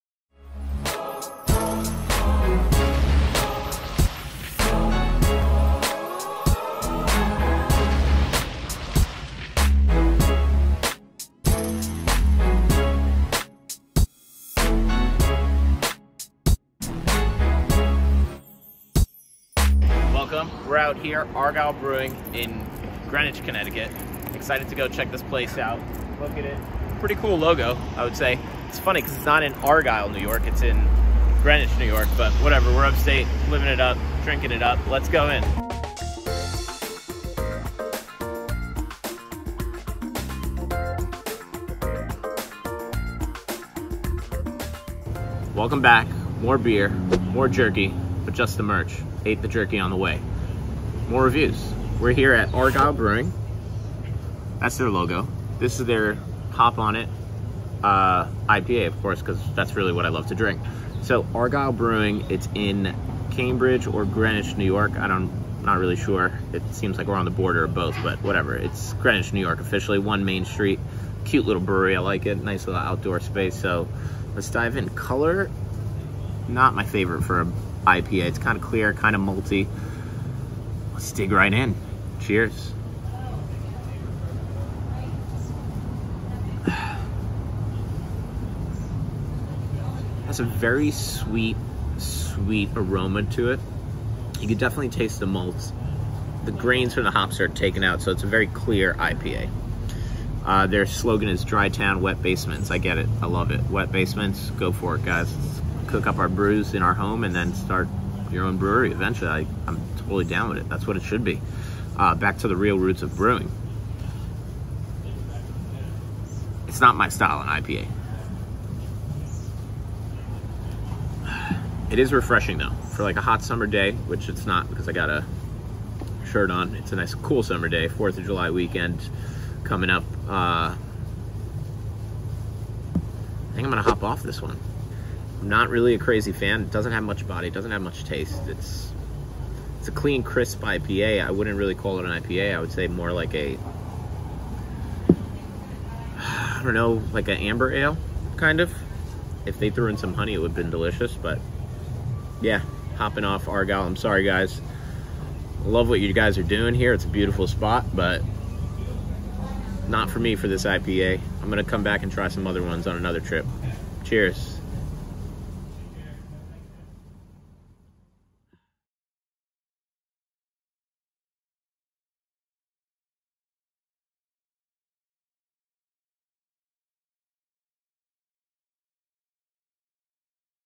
BEER BITES n' BREW's Beer Review of Argyle Brewing Co - Hop On it IPA
BEERBITESBREW.com Go Check Us Out.

Sipping on some Argyle Brewing Hop On It IPA… Drink Town Wet Basement! let’s start drinking. This is a refreshing beer, however it is very light for an IPA. More of a West Coast IPA, but not a North Easy Hoppy Hazy IPA.

ABV - 5.3%

IBU - 67

Untappd - 2.25